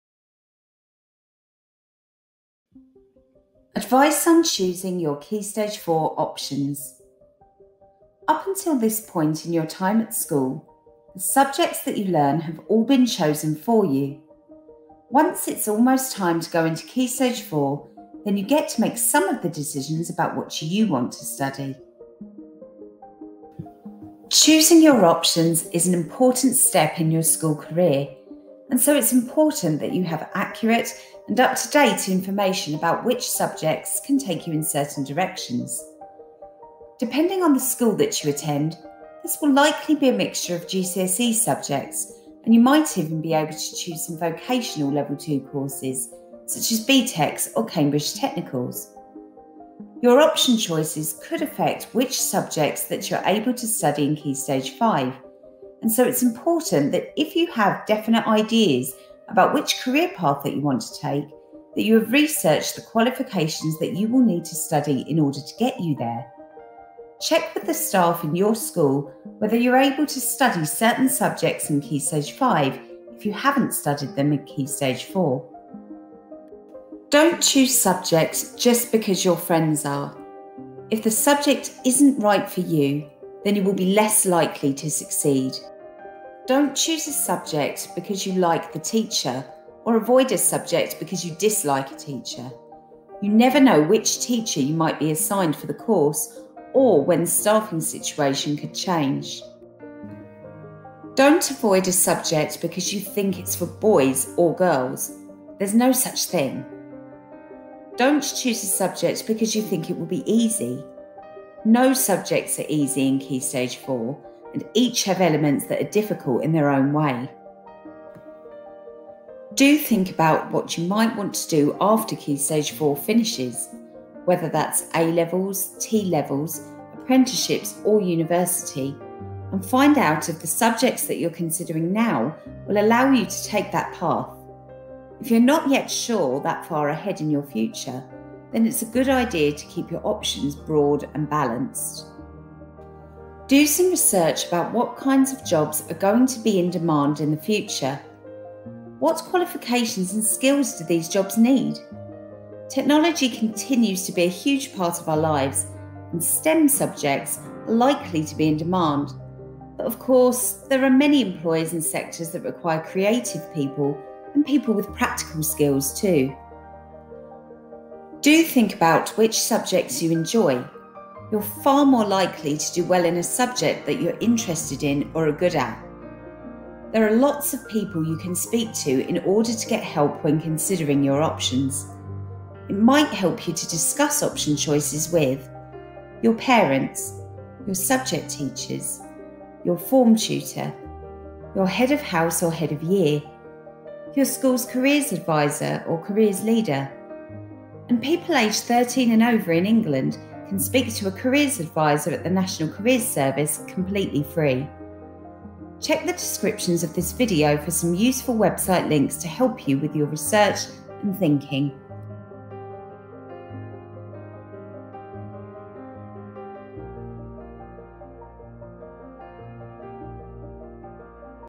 Advice on Choosing Your Key Stage 4 Options
Advice about choosing your GCSE/Level 2 options for Key Stage 4.

What's the Point? Careers